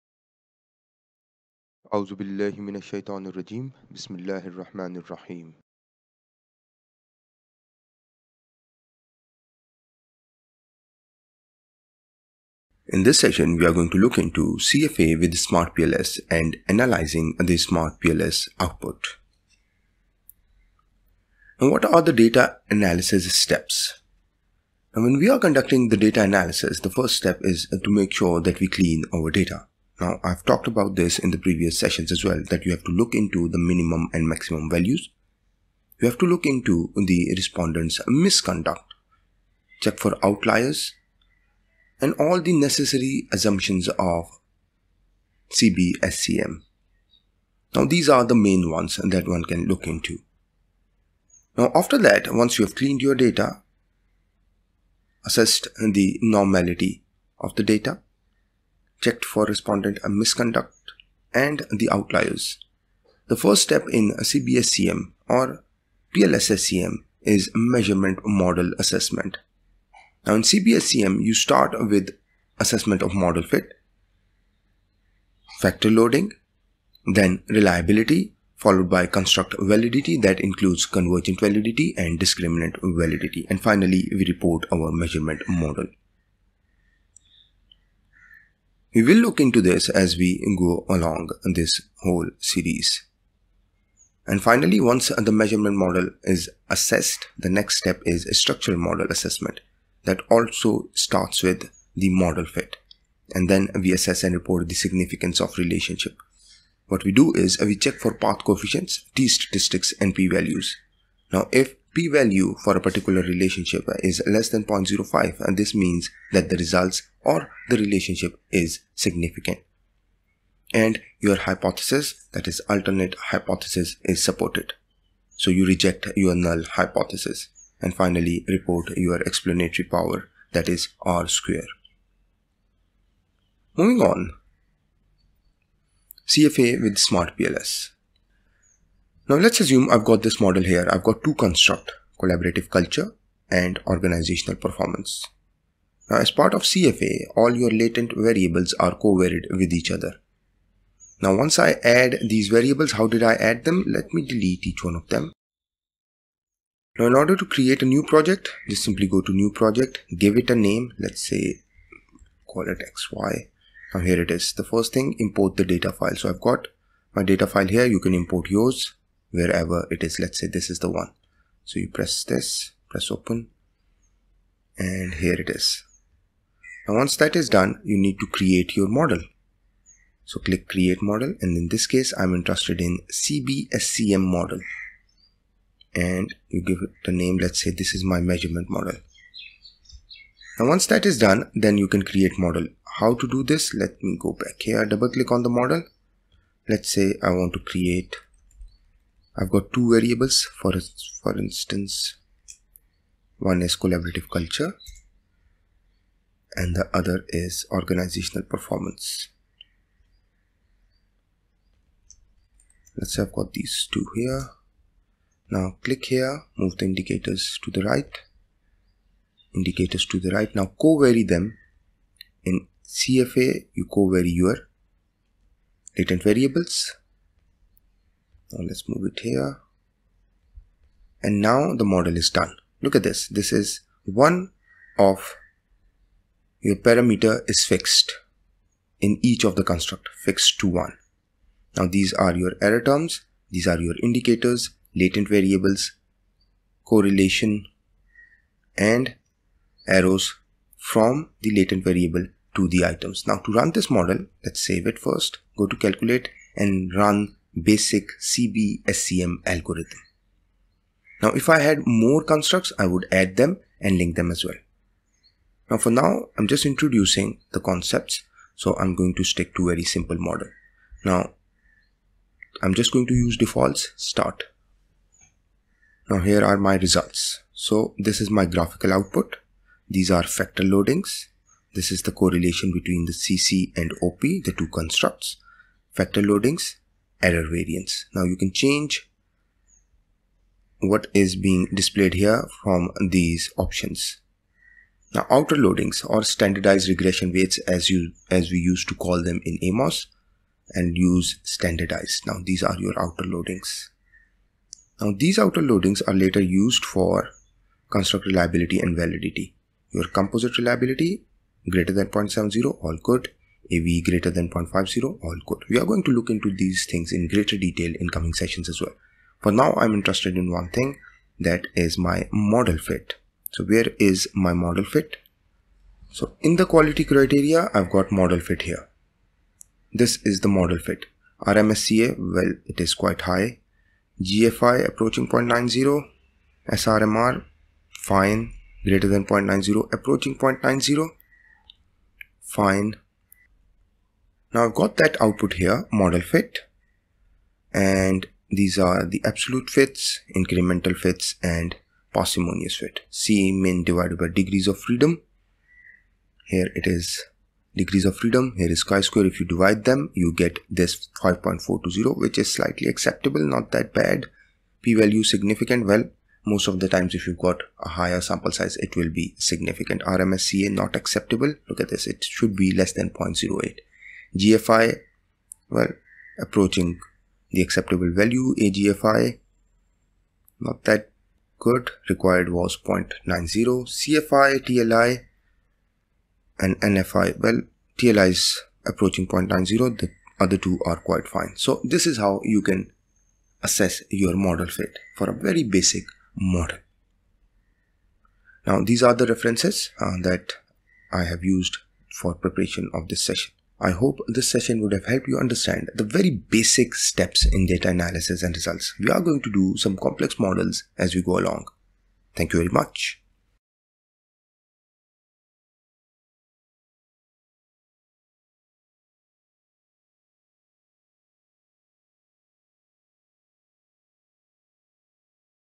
CBSEM using SmartPLS4 | 7 | Basic CFA Model with SmartPLS
As part of the series on CB-SEM using SmartPLS4, the focus of the session is to design a basic model in SmartPLS and develop a basic understanding of factor loadings and assess the model fit statistics.

The session discusses in detail the concept of factor loadings, the threshold, the concept of fit statistics, the common fit statistics, interpreting the fit statistics, and the threshold of fit statistics.